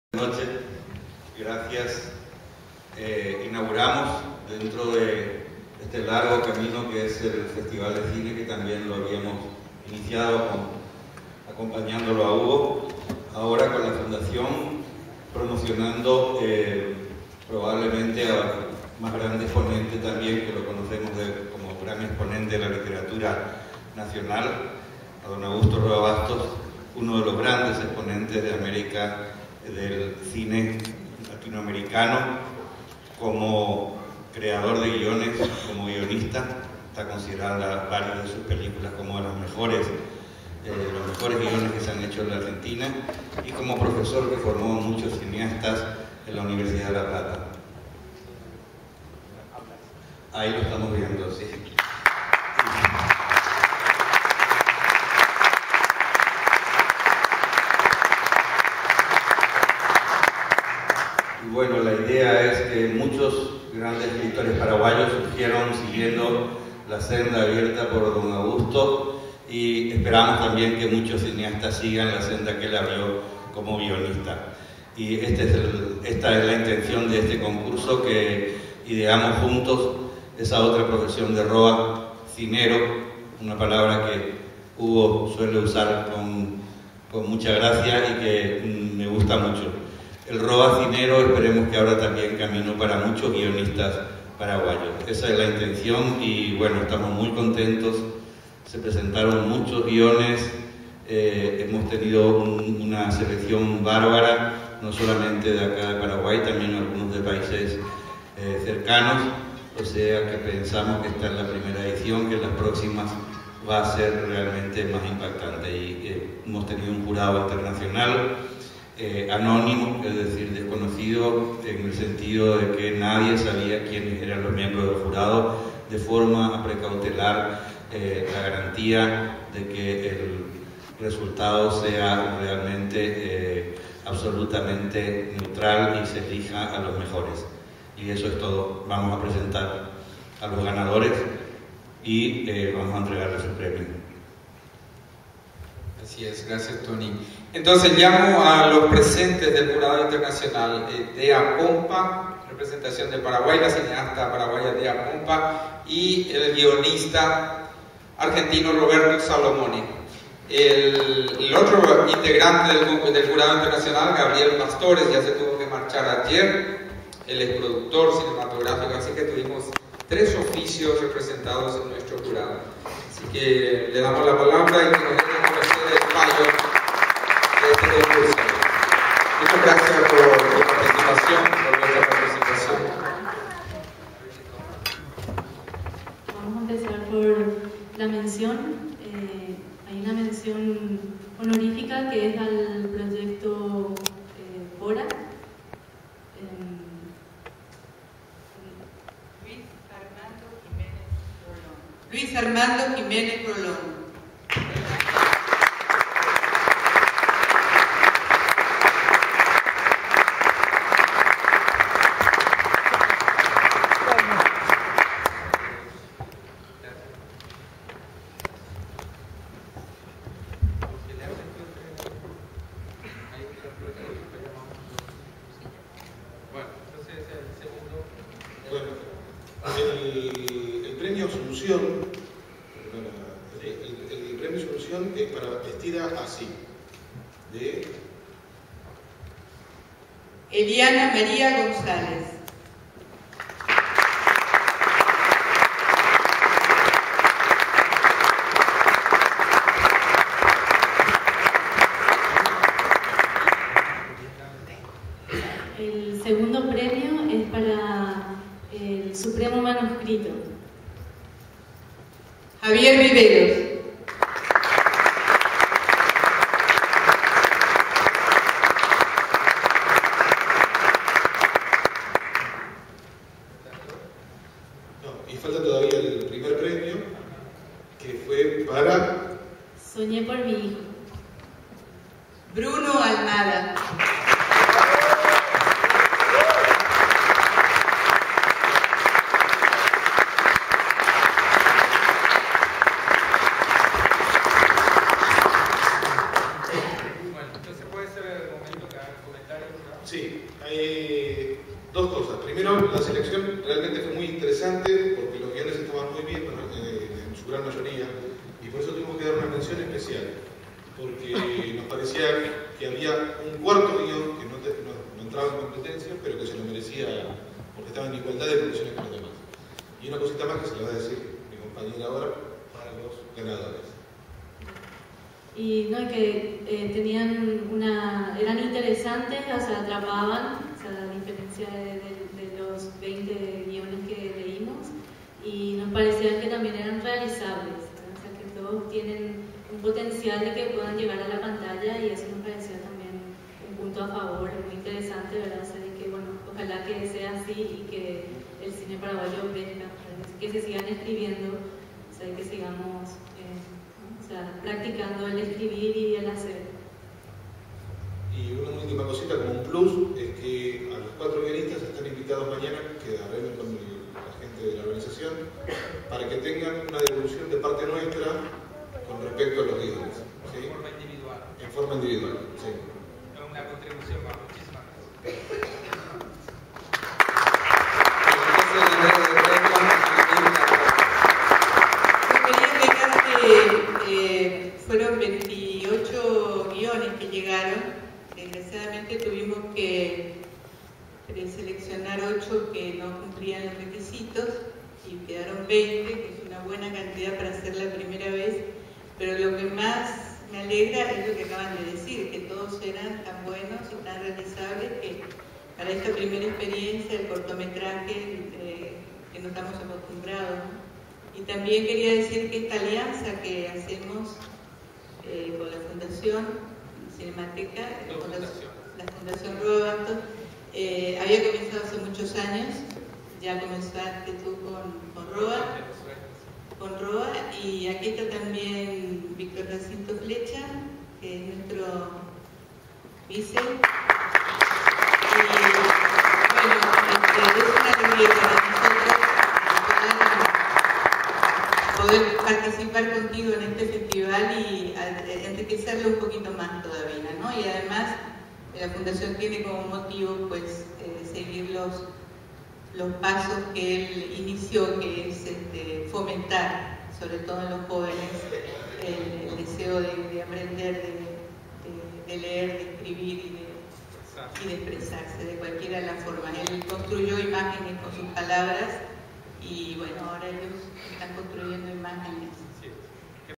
Clausura 22 Festival Cine Paraguay - Concurso de Guion Roa Cinero
Teatro Municipal de Asuncion. 30 de setiembre de 2013.